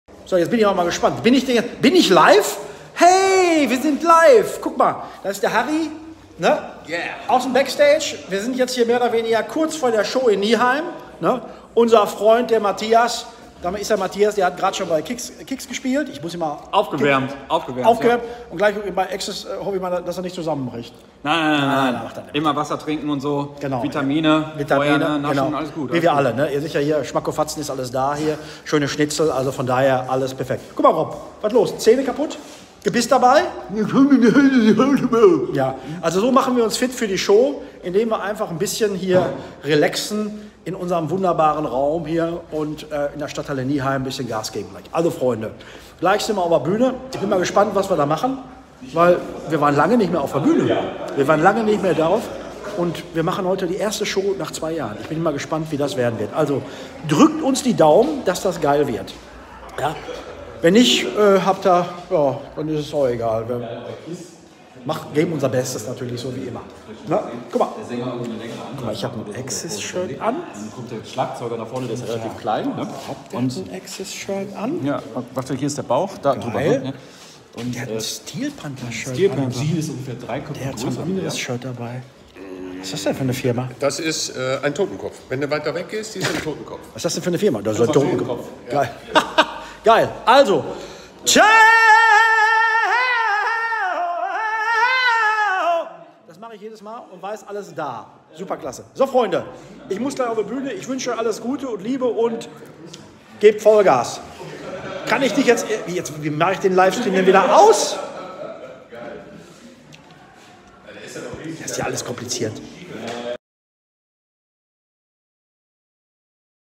AXXIS Backstage: right before the show in Nieheim / Stadthalle
AXXIS im Backstage der Stadthalle Nieheim. Kurz bevor die Show startet.

@axxisworld  Axxis - Topic